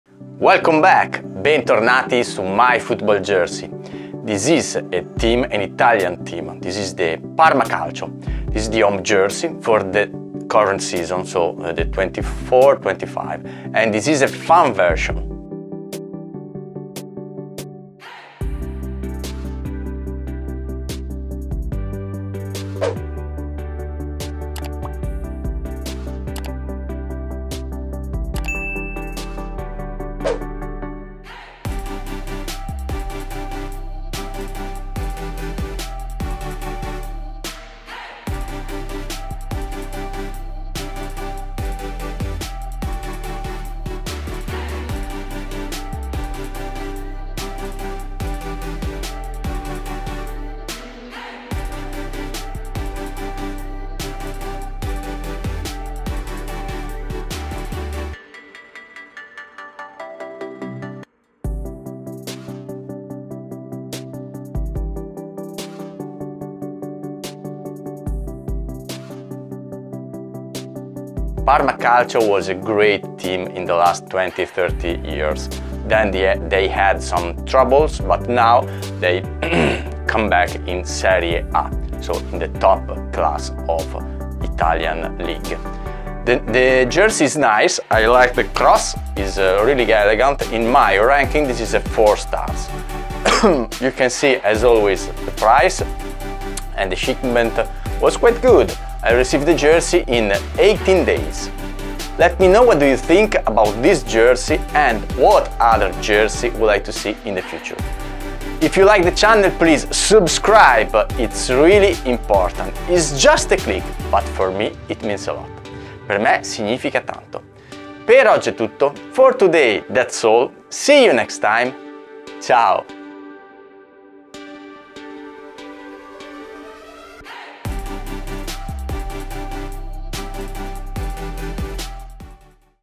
Parma Calcio Home Jersey 24/25 (Minejerseys) Fan Version Unboxing Review
Discover the Parma Calcio Home Jersey 24/25 (Fan Version)
Watch till the end to see how the Parma Calcio Home Jersey 24/25 (Fan Version) fits!
If for some reason the link for the Parma Calcio Home Jersey 24/25 doesn't work, write a comment about it, I will reply as soon as possible.

The Parma Calcio Home Jersey 24/25 (Fan Version) costs only 26.99 $! ** Shipping INCLUDED **

Find out the measurements of the Parma Calcio Home Jersey 24/25 (Fan Version).
Size: Medium
Jersey Width: 53 cm
Jersey Length: 73 cm
Model Height: 175 cm (5 ft 9 in)
Model Weight: 73 Kg (161 lbs)

Timecodes
00:00 Overview
00:22 Details View
01:11 Pricing
02:04 Dimensions & Try On

Please watch the Parma Calcio Home Jersey 24/25 (Fan Version) video review in 1080p (HD)